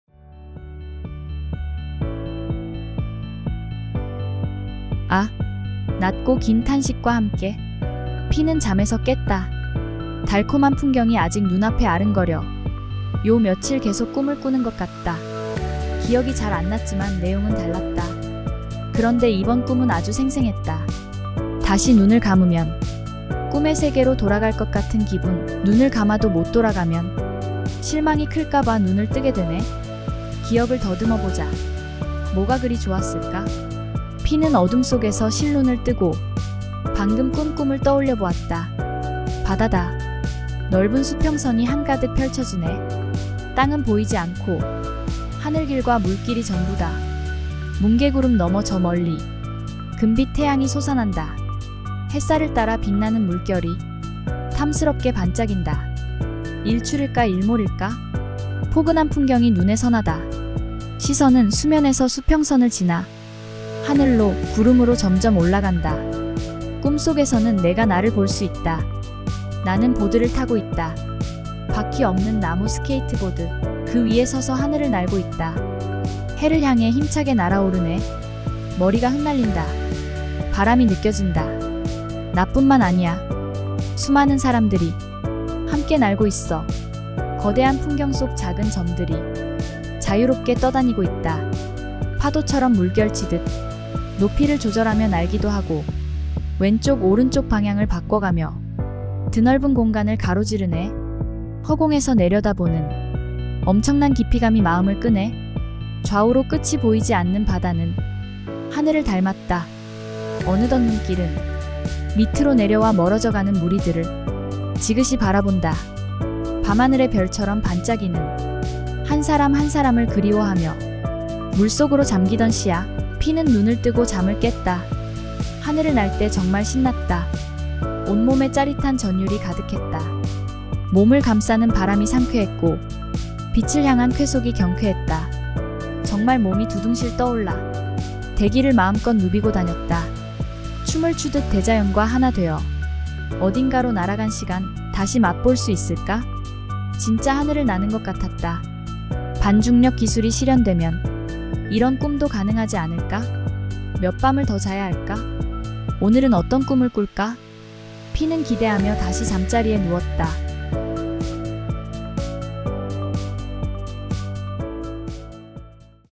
[ 7/16 - 에세이] '하늘을 날아다니는 꿈'
매일 브런치스토리에 올리는 글을
영상으로 만들었습니다.

『365일 작가연습』 이라는 책을 중심으로
매일의 주제에 맞춰 글을 쓰고 있습니다.
7/16일의 주제는 '하늘을 날아다니는 꿈' 입니다.
감사합니다.